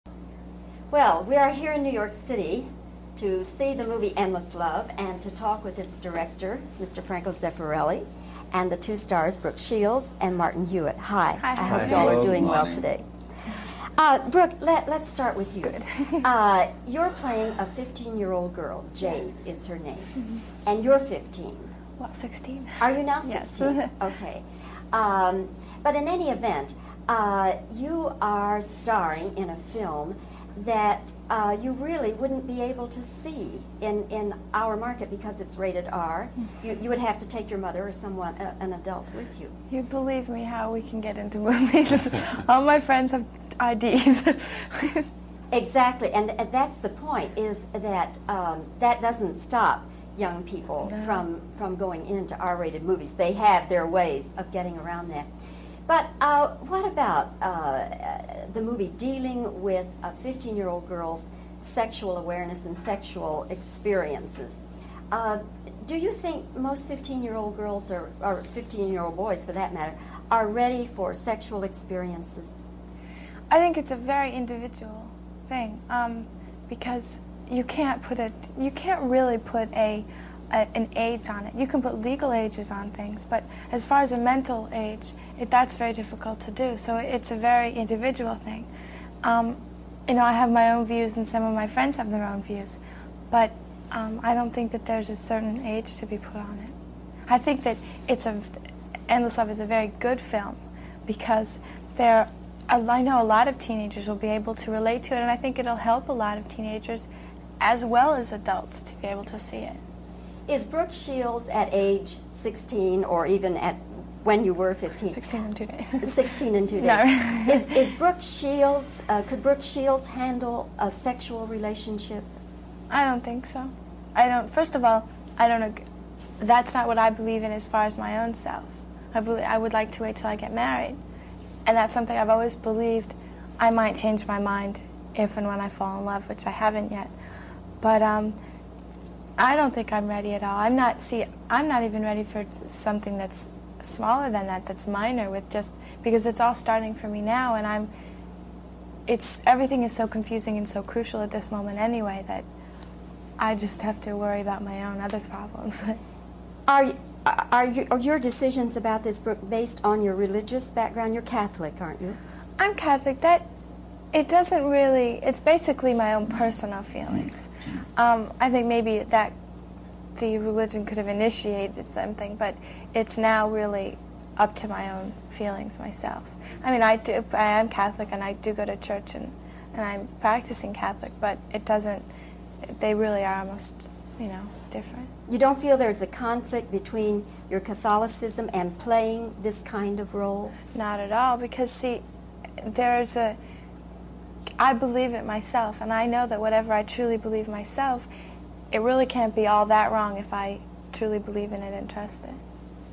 16 year old Brooke Shields interview
Bobbie Wygant interviews Brooke Shields for the movie Endless Love in 1981